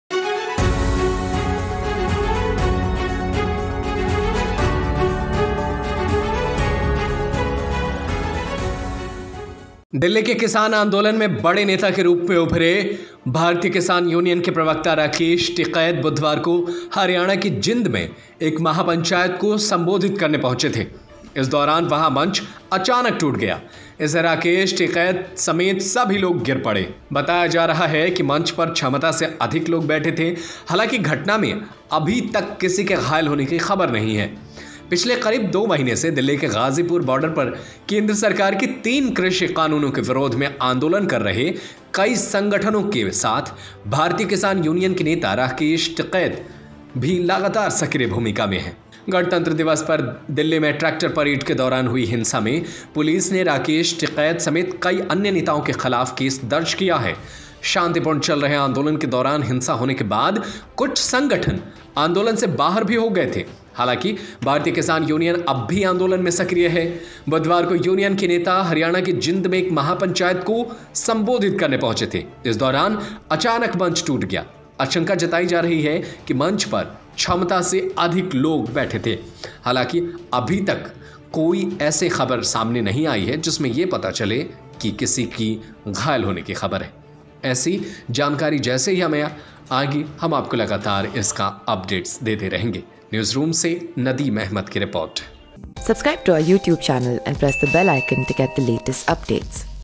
वीडियो महापंचायत में टूटा मंच, राकेश टिकैत सहित कई नेता गिरे
Repoter : Md Nadim
News Editor By : Sandeep Arya